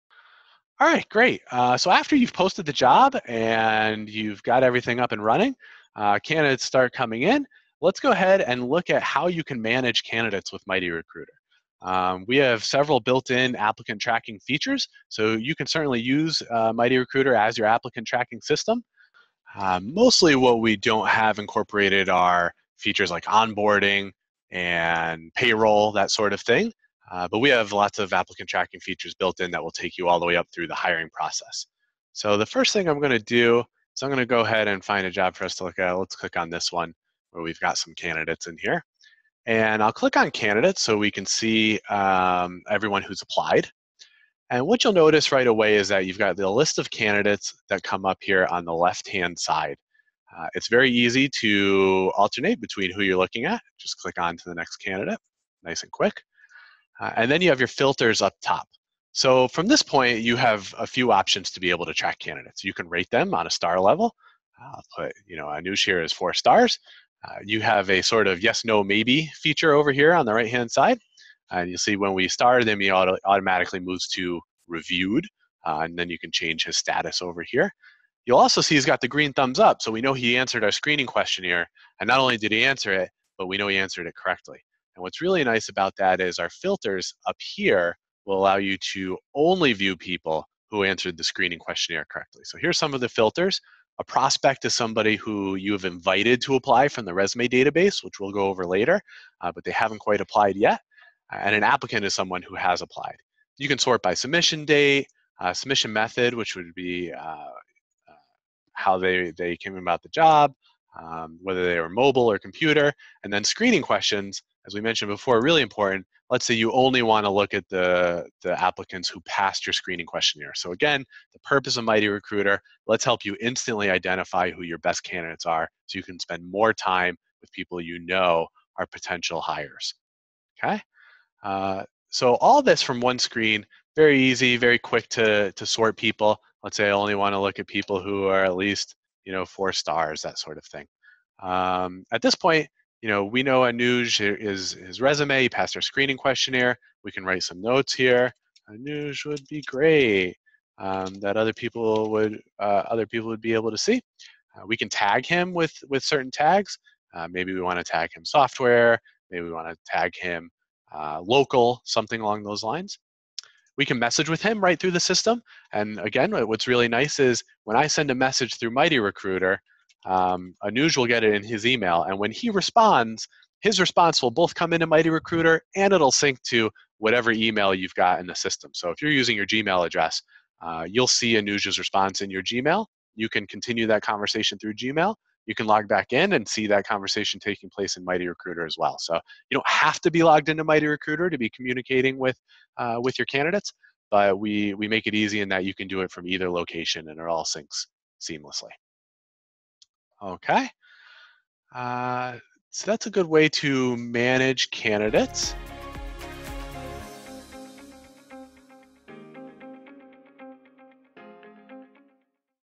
Applicant Tracking Features (ATS): How to Manage Candidates on MightyRecruiter
After you’ve completed your job postings, let’s take a look at how you can manage candidates on MightyRecruiter. We have several built in Applicant Tracking Features you can use to optimize your recruiting process. Manage candidates with filters like distance, rating, application content and more.

Make your recruiting job easier and better with MightyRecruiter! Create your job posting today